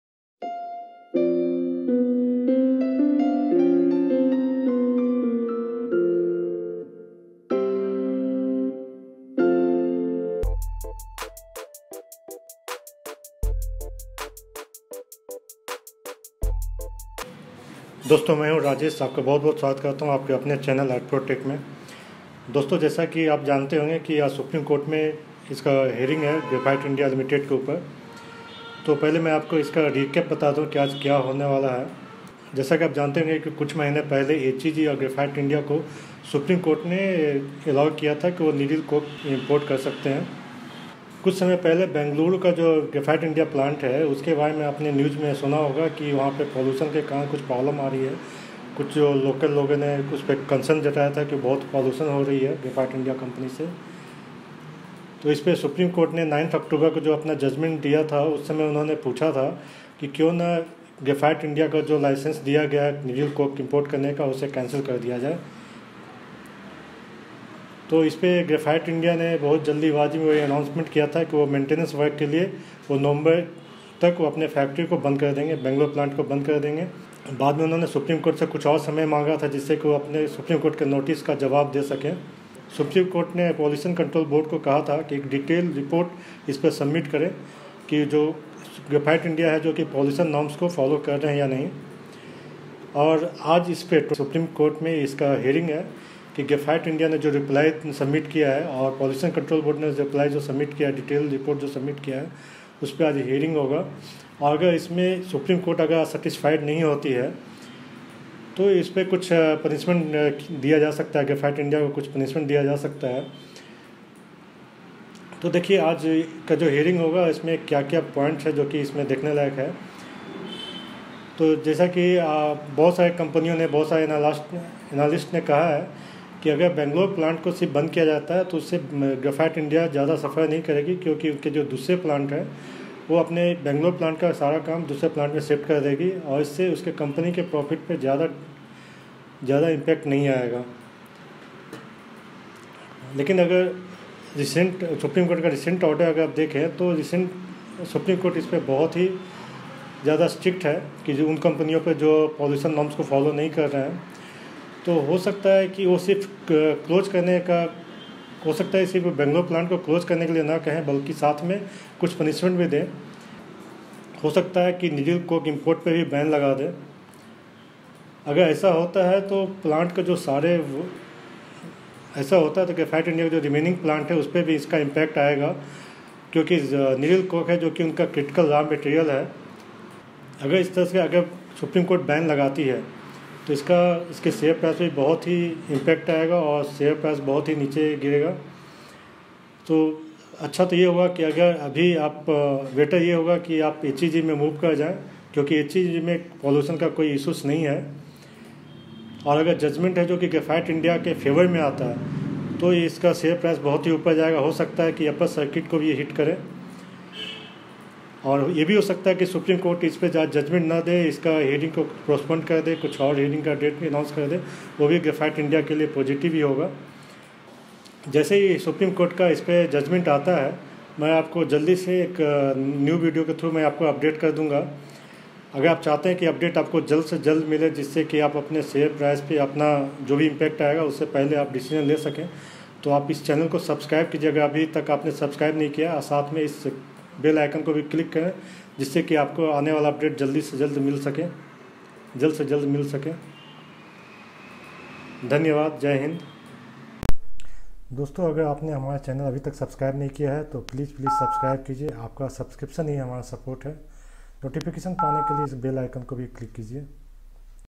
Supreme Court hearing on Graphite India on Oct 23rd 2018
Friends, as you might be aware Graphite India and HEG has been granted needle coke import by Supreme Court a month back. After that it appeared that all the problems with import has been solved for these two companies. But after that Graphite India has been in news for its Bengaluru plant for causing pollution in neighborhood. The residents of Whitefield area in Bengaluru have been relentless in their pressure on the company for fixing the issue. Later company has given many clarifications on the plant. But on Oct 9th when SC was hearing the pet coke ban issue for Aluminium and Calcining industry, pollution control board has complained regarding this issue. SC then asked Graphite India to submit a reply to court on why not there needle coke import license can be cancelled because of this. After that Graphite India in a hurry announced that they will be closing the plant till Nov 2018 to carry out the maintenance work to fix the issues found in inspection. Graphite India has sought more time to reply to that notice. After that SC has asked pollution control board to submit the report detailing the violations on pollution norms.
So today on 23rd SC will be hearing the Graphite India reply as well the report from pollution control board. If SC is not satisfied with the reply and the report is also negative regarding pollution level, they may punish Graphite India severally.

Equipment I use:
1. Mic for Audio Recording
2. Smartphone for Video Recording
3. Tripod for holding Smartphone
4. Background Red Screen
5. Laptop
6. Youtube Tshirt